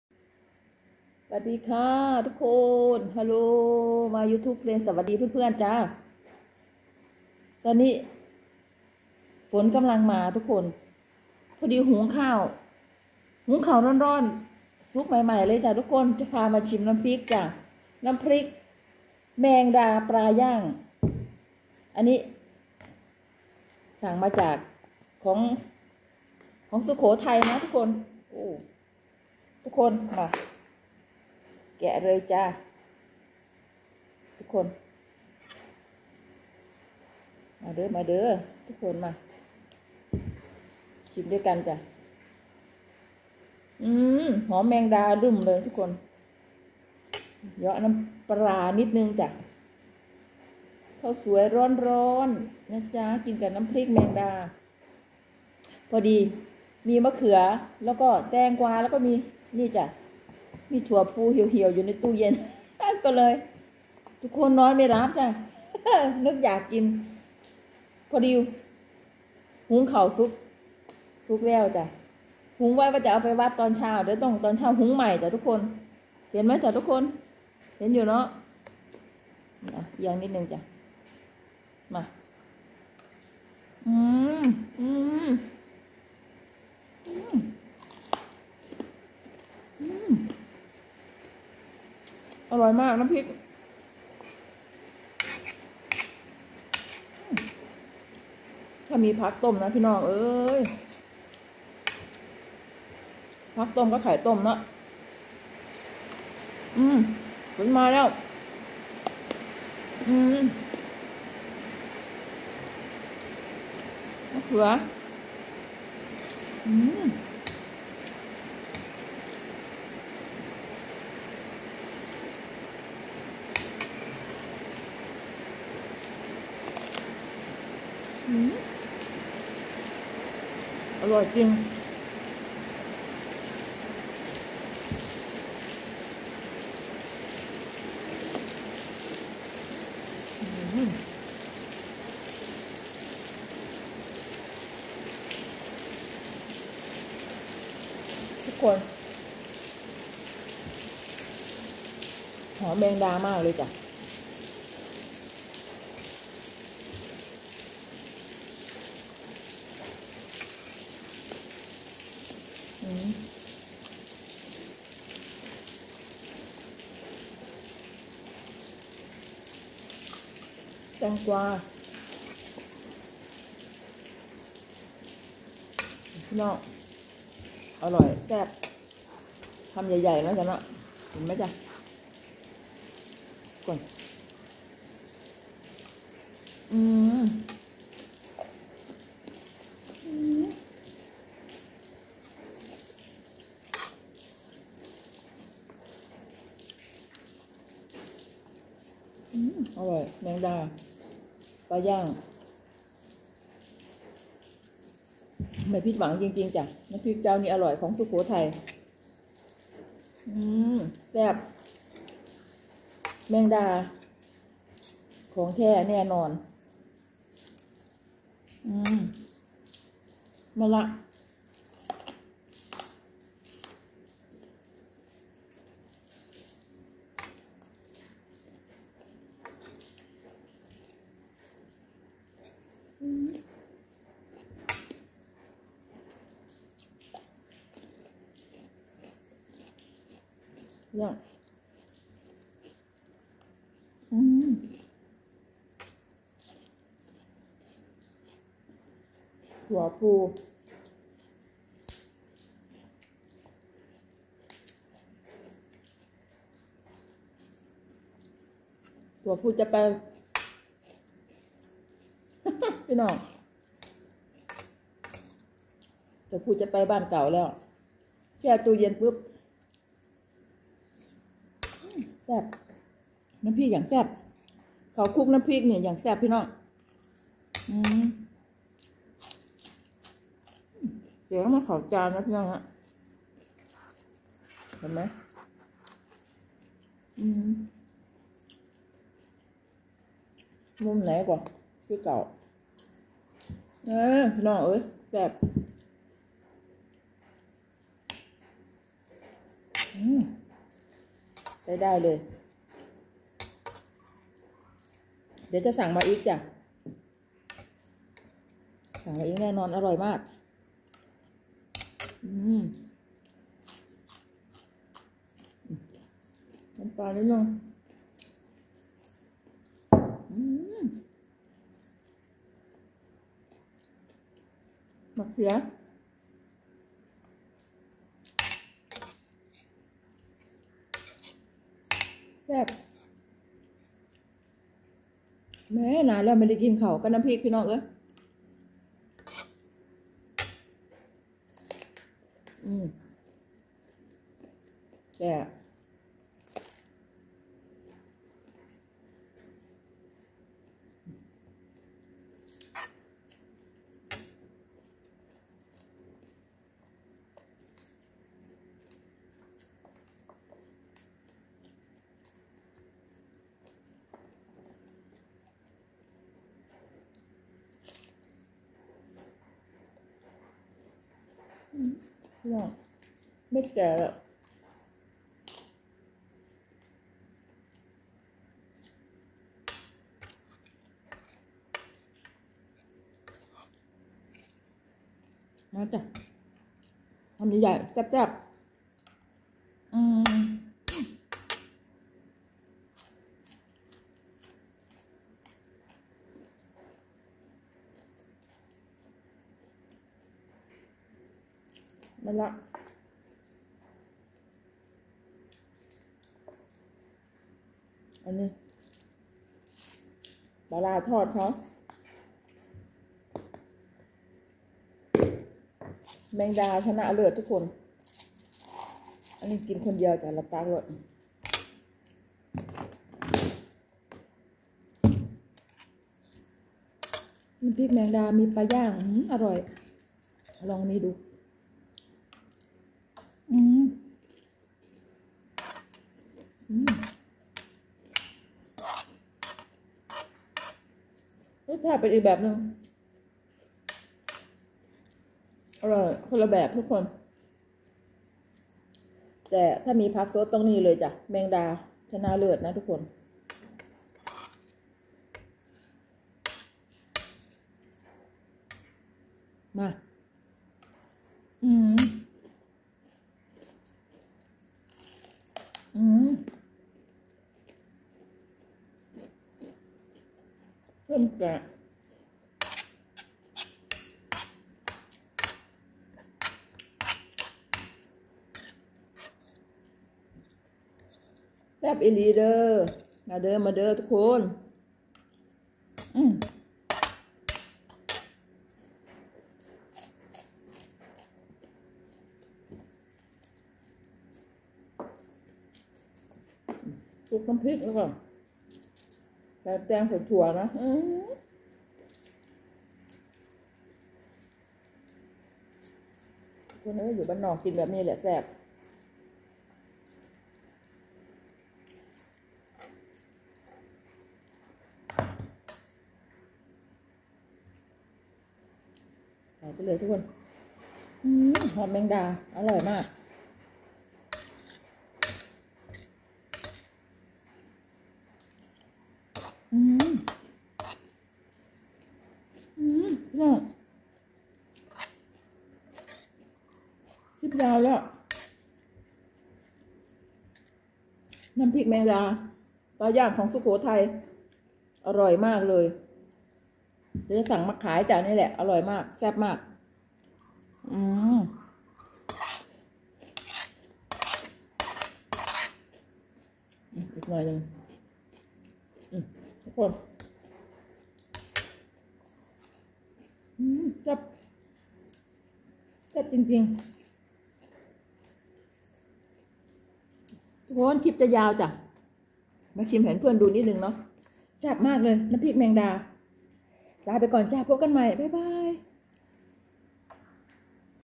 ข้าวสวยร้อนๆ คลุกน้ำพริกแมงดาปลาย่าง แซ่บ สูตรสุโขทัย
น้ำพริกแมงดา ปลาย่าง